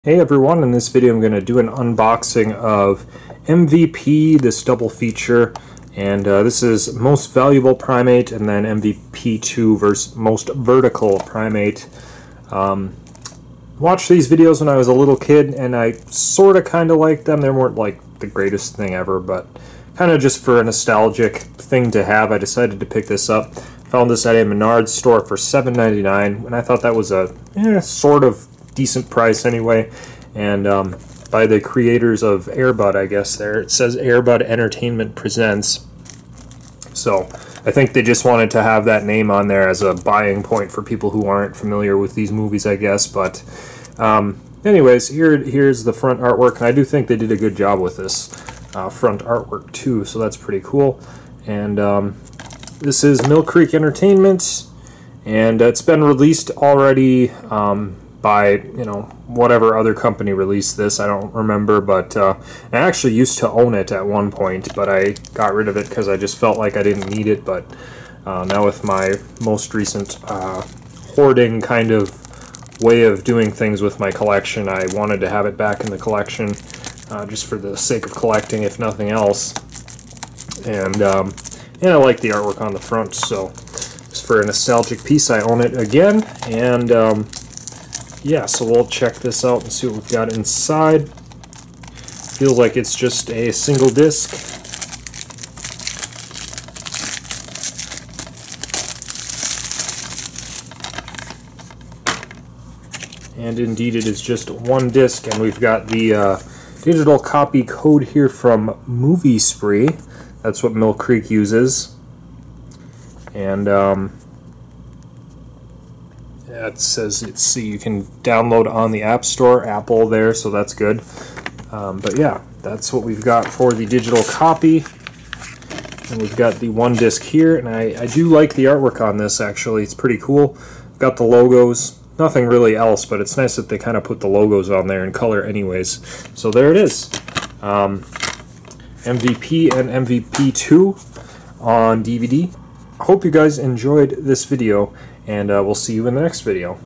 MVP 1 & 2 Most Valuable Primate and Most Vertical Primate Double Feature DVD Unboxing
Here's an unboxing of MVP 1 Most Valuable Primate and MVP 2 Most Vertical Primate Double Feature on DVD.